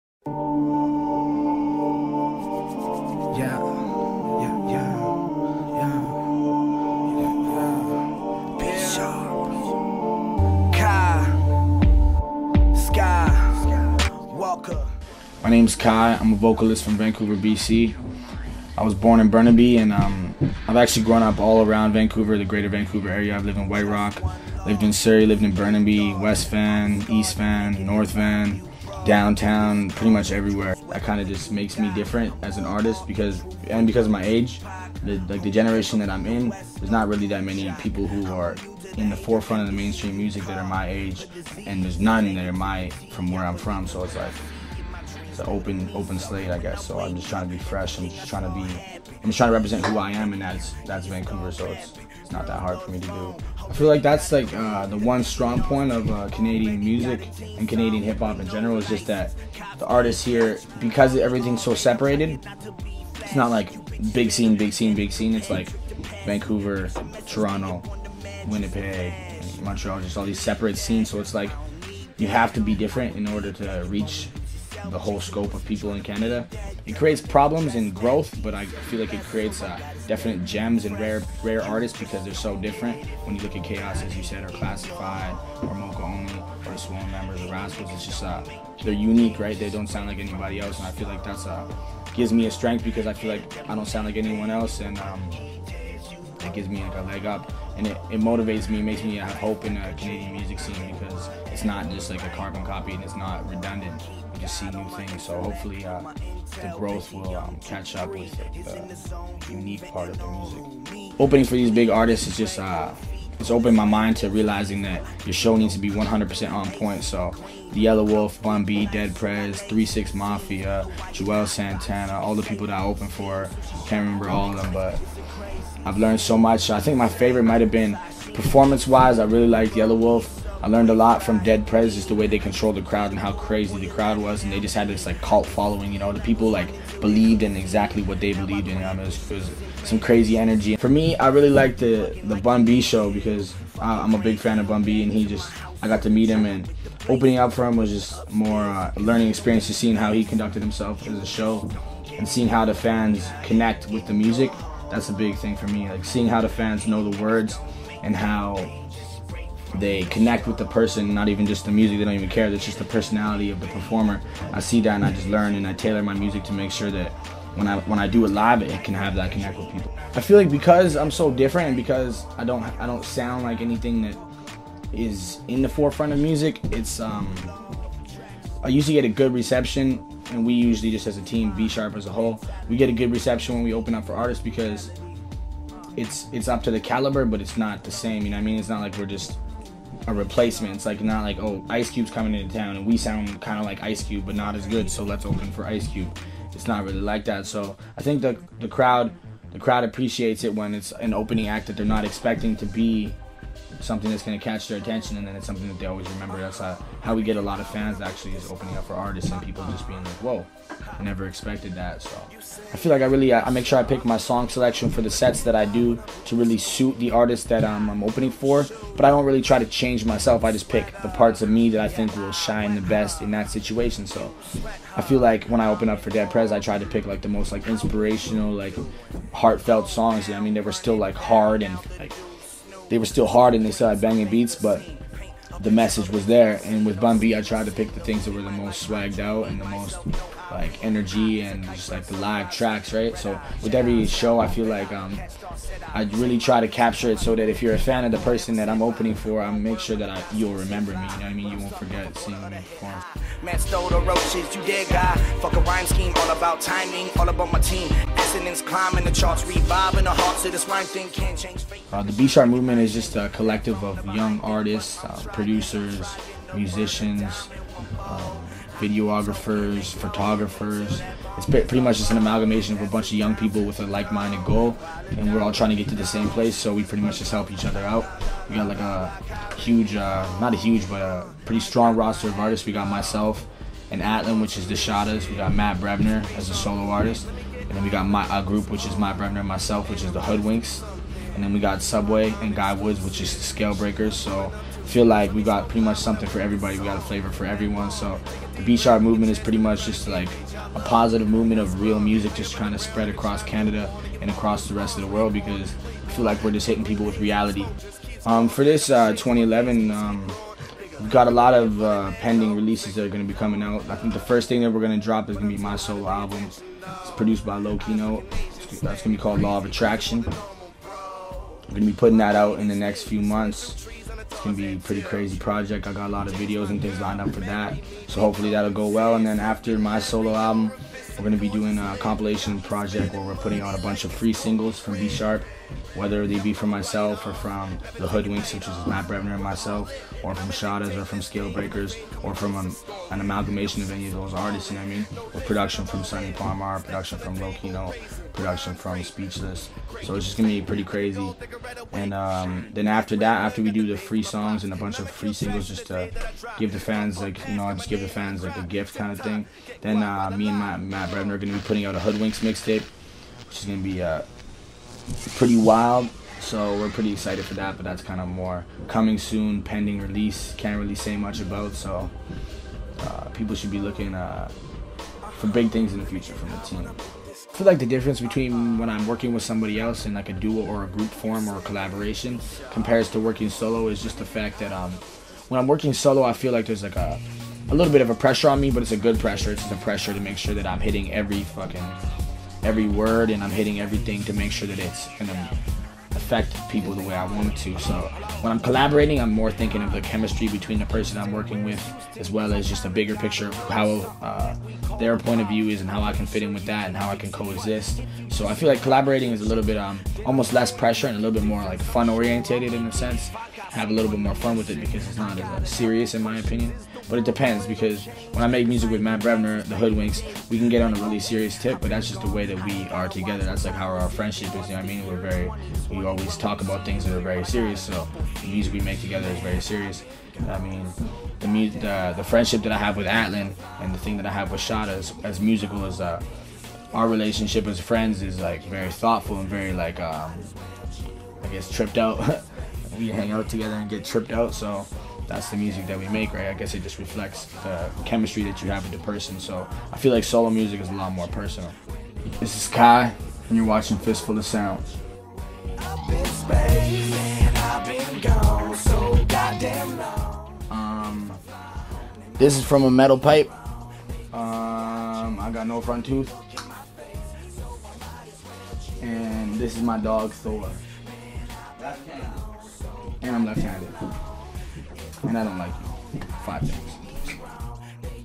Interview with KAi Sky Walker
We had the privilege to chat with KAi Sky Walker in his home about opening up for bigger artists, how he stands out from other Canadian hip hop artists, the B Sharp Movement and so much more!

Keep an eye out for KAi in the near future. Stay updated by checking out his Facebook

Interview by: Jan Galvez
Filmed by: Julian Fok
Edited by: Jan Galvez